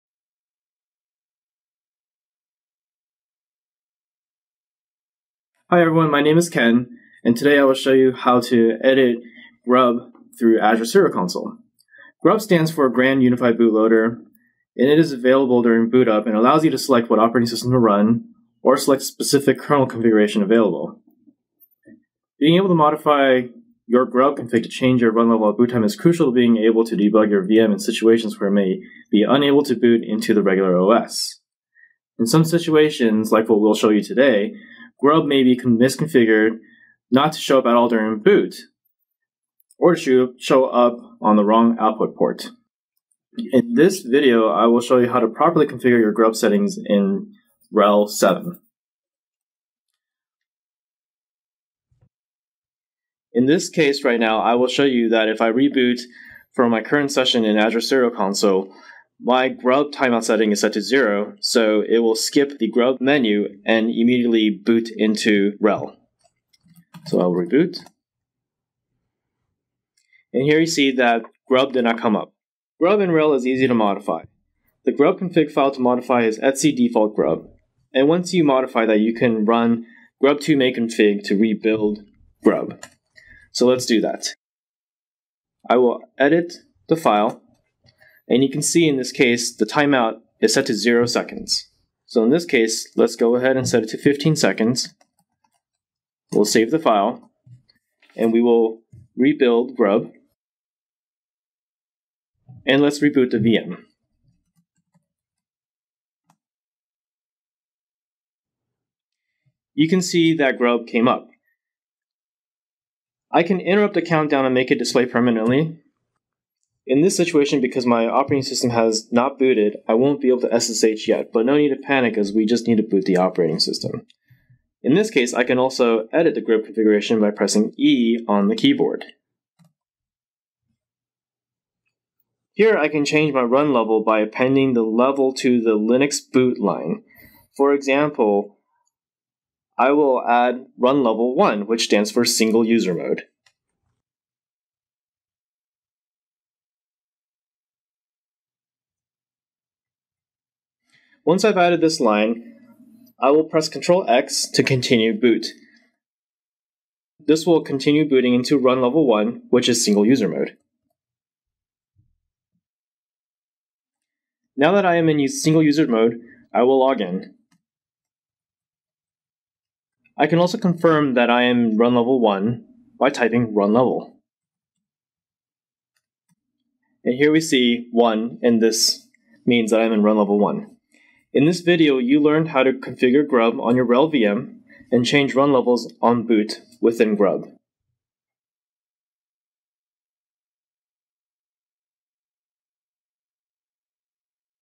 How to use GRUB and the Azure Serial Console on a Linux Virtual Machine | Azure Tips and Tricks
In this video of the Azure Tips and Tricks series, Ken from the Azure Serial Console team will show you how to enable GRUB on your RHEL VM. You will also learn how to use GRUB to enter single user mode.

Get started with 12 months of free services and $200 USD in credit. Create your free account today with Microsoft Azure